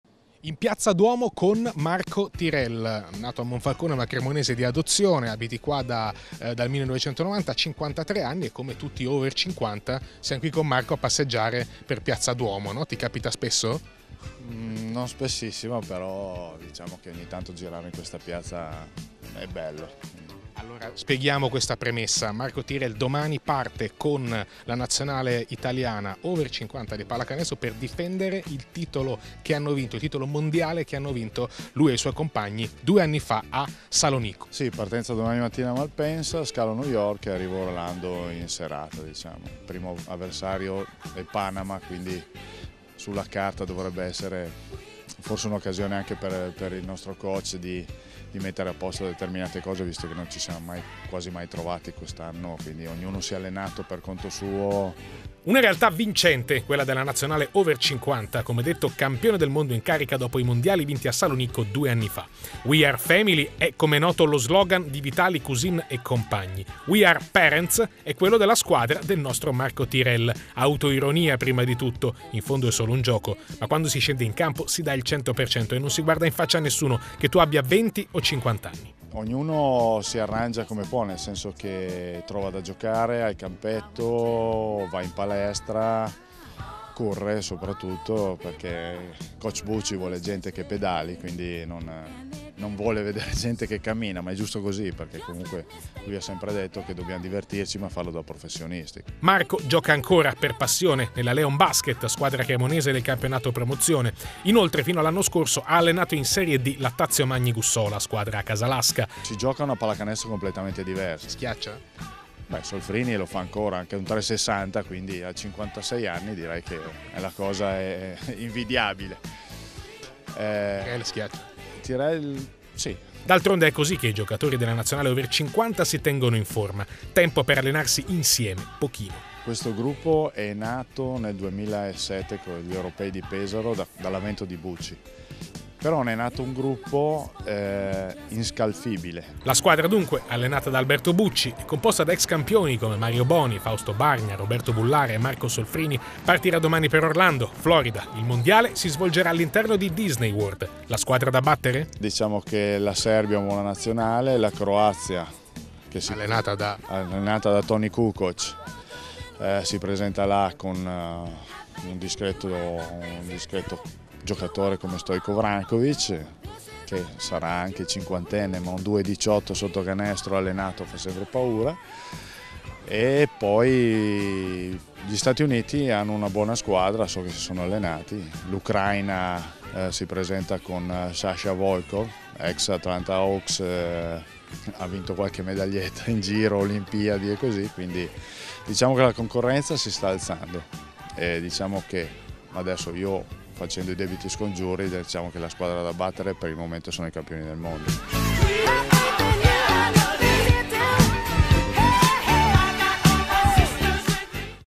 Mondiale di basket Over 50 a Orlando, Florida: l'Italia c'è, Cremona pure, con Marco Tirel
Il servizio di Giacomo Iacomino